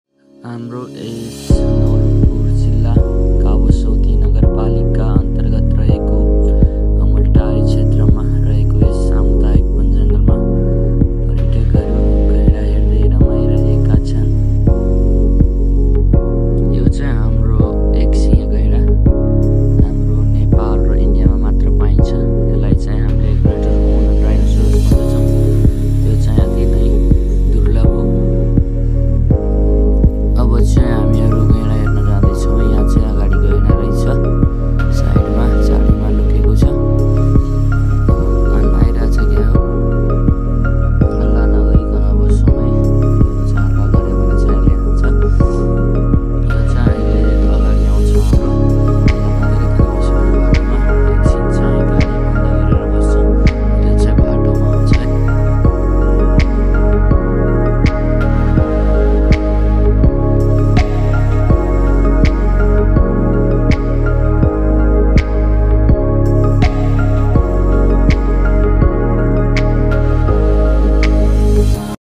JUNGLE SAFARI
जंगल सफारीमा पर्यटकहरू गैडां संग रमाउदै । अवस्य घुम्न आउनु होला। सम्पर्क नं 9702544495